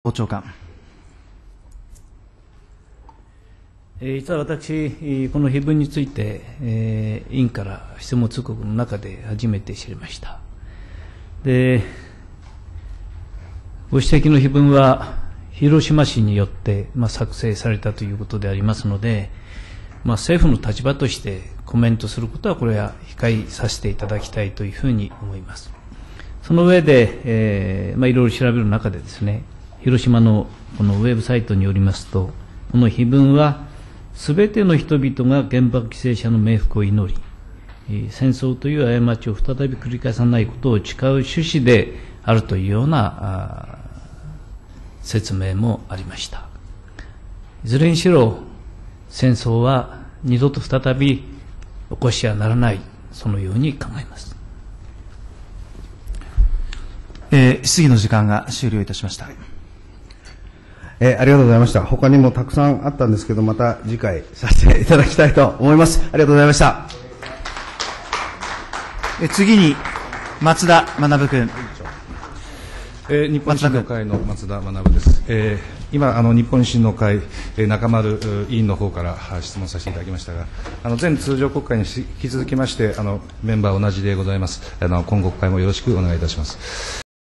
【オバマ広島5/27記念】菅義偉64才で 「あやまちは繰返しませぬから」の碑文を知らなかった？え？
追記【オバマ5/27/2016広島】2013/11/6 衆院内閣委員会 菅義偉官房長官の原爆死没者慰霊碑「碑文を知らなかった？」質問通告で「はじめて知った」の驚き　【全３分→】 中丸啓(維新)の質疑 「安らかに眠って下さい 過ちは 繰返しませぬから」の「あやまち」の文言を広島からなくしたい2014年衆院選で落選
486　5/25